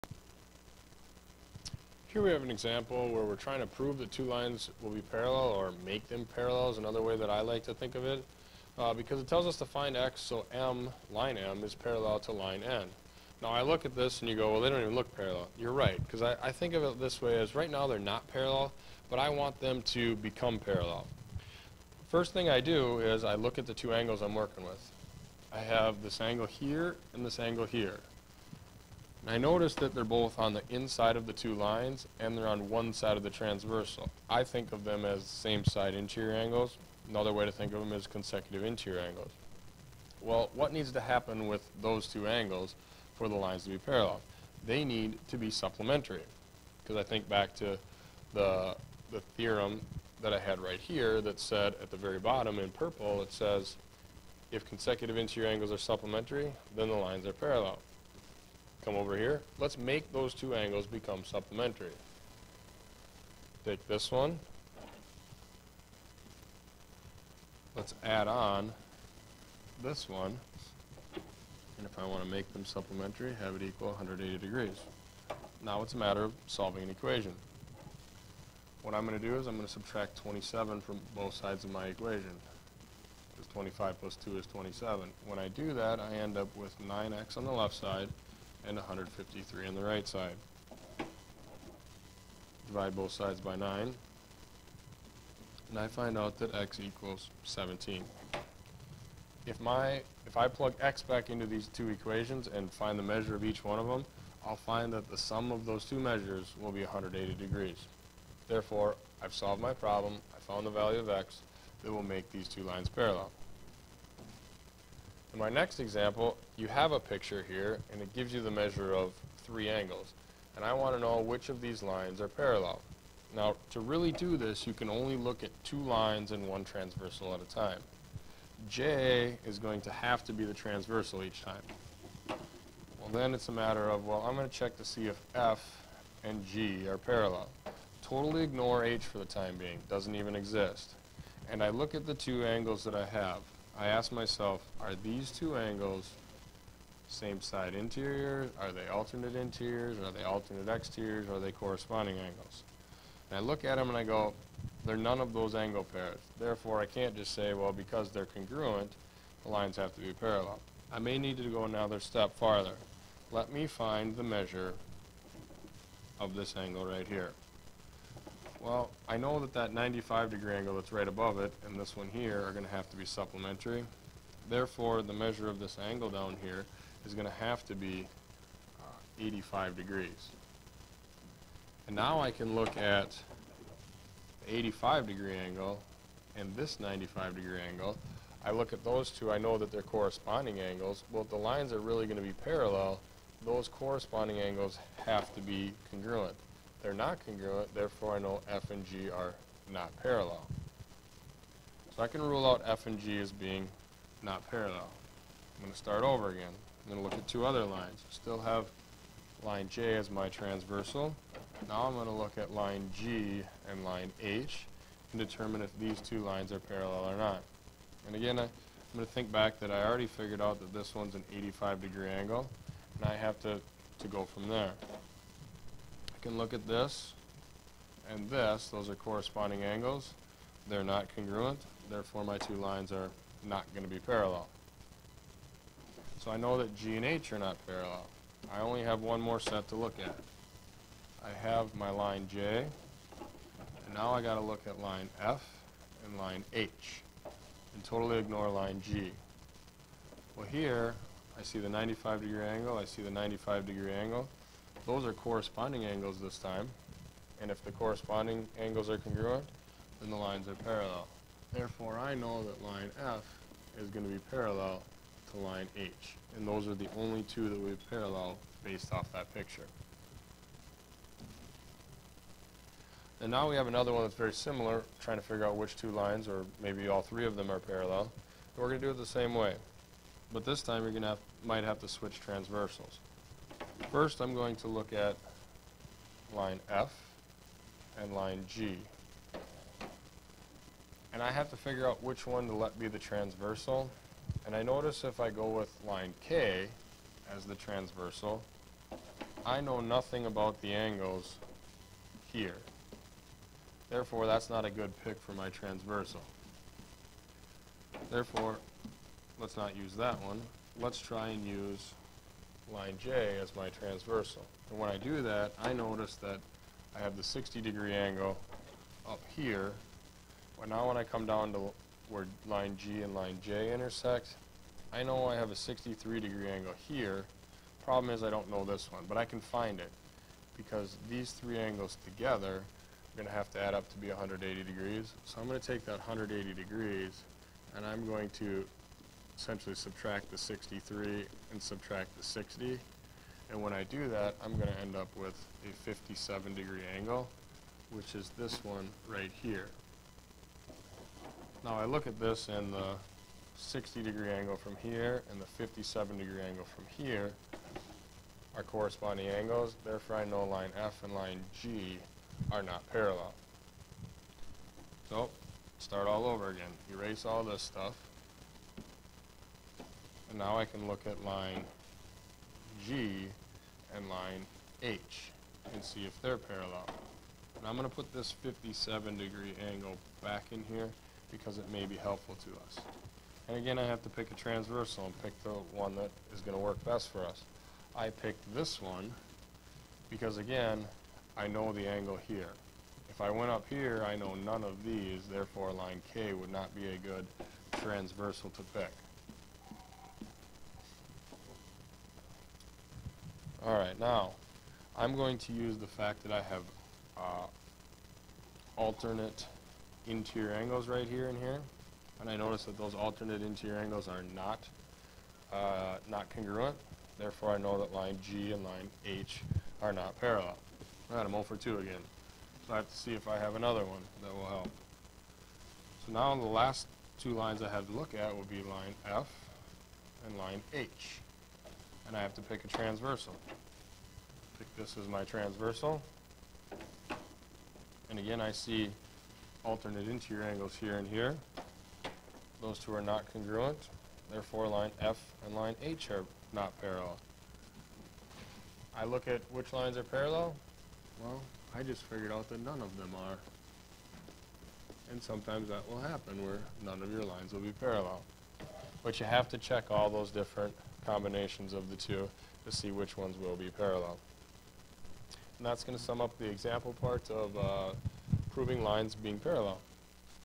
3 5 Proving Lines Parallel Exampes
One example showing how to find a value of "x" which will make two lines parallel and two examples showing how to use different angle pairs to determine if lines are parallel.